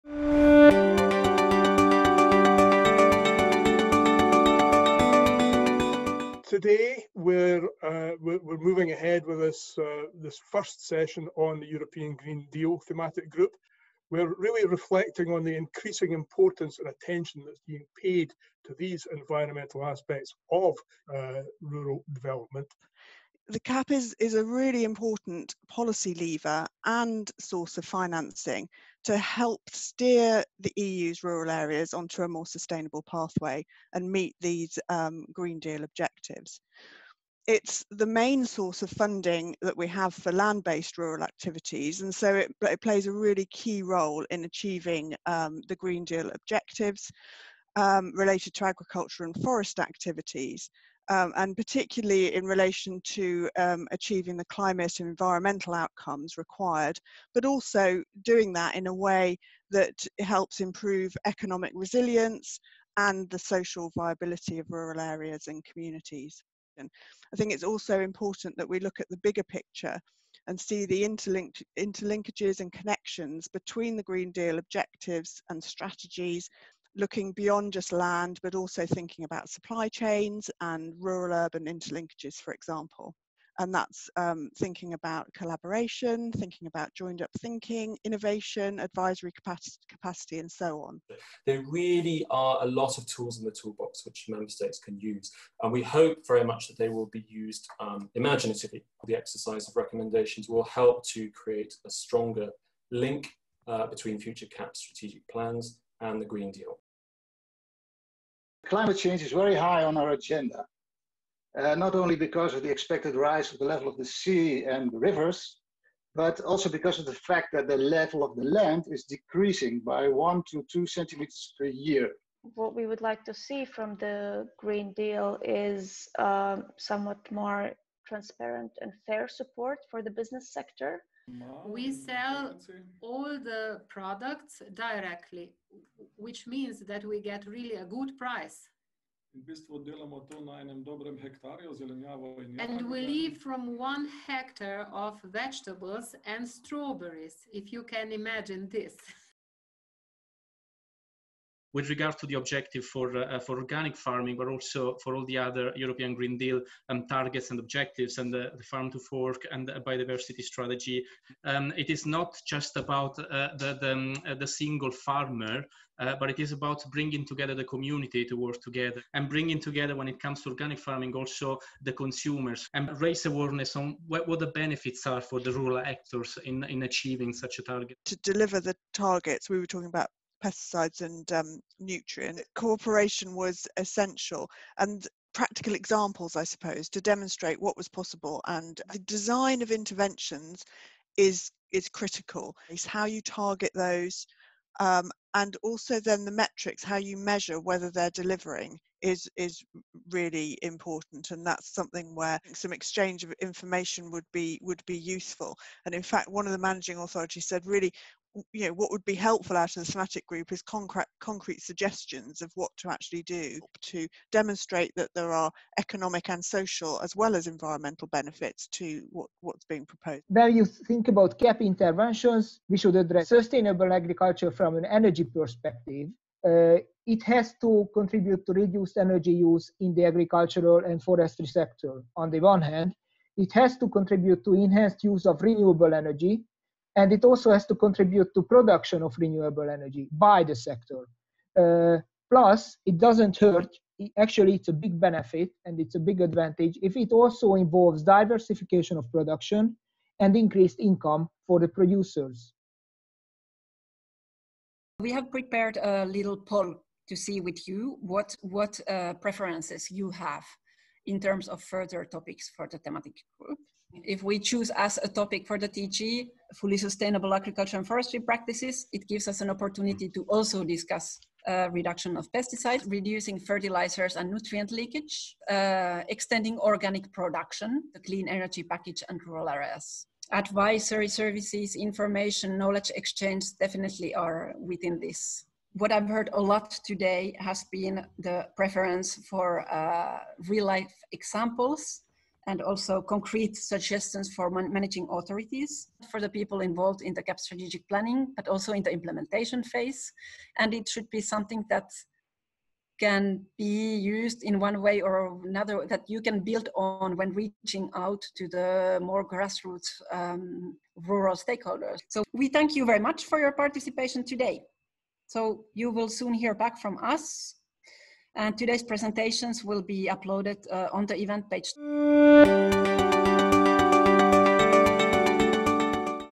European Green Deal and Rural Areas: ENRD Thematic Group 1st meeting
The ENRD Thematic Group (TG) on ‘European Green Deal and Rural Areas’ (EGD) focused on the EGD’s role in rural areas and on rural and agricultural activities’ possible contributions to achieving selected EGD objectives. The TG’s first meeting defined the specific topics that the TG will address in its further work until May 2021.